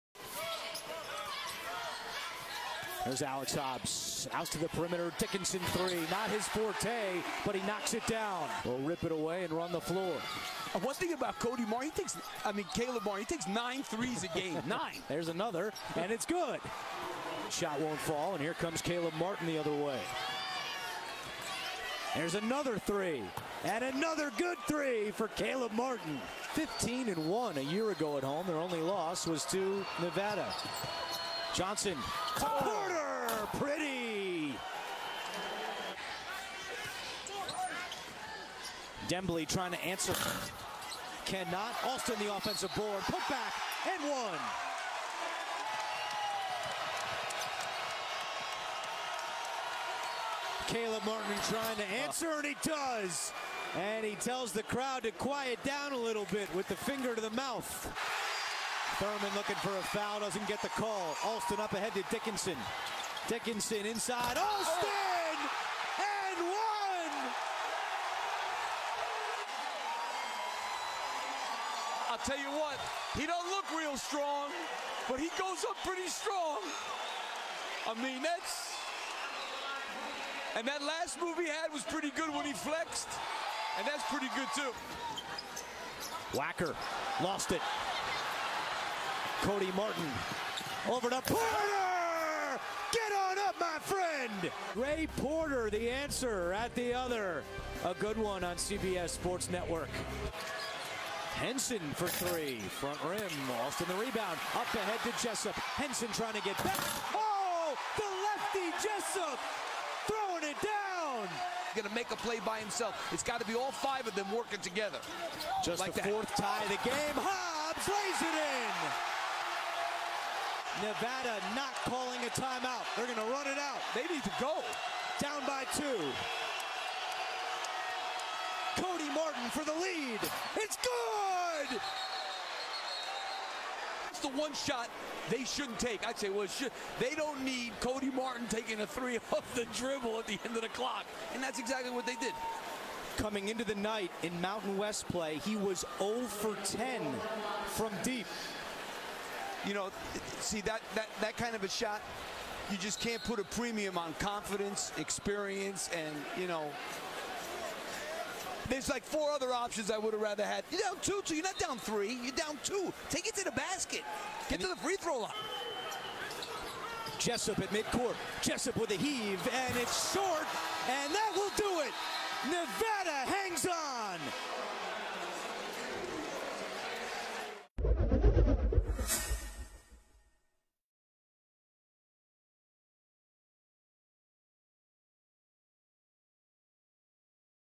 No. 10 Nevada vs. Boise State Basketball Highlights (2018-19) | Stadium
#10 Nevada vs. Boise State basketball highlights from 2019 game. Cody Martin tallied 16 points, including the go-ahead 3-pointer in the final seconds, to help the Wolfpack avoid an upset with a 72-71 win over Boise State. Jordan Caroline added 15 points and 13 rebounds for Nevada in the win.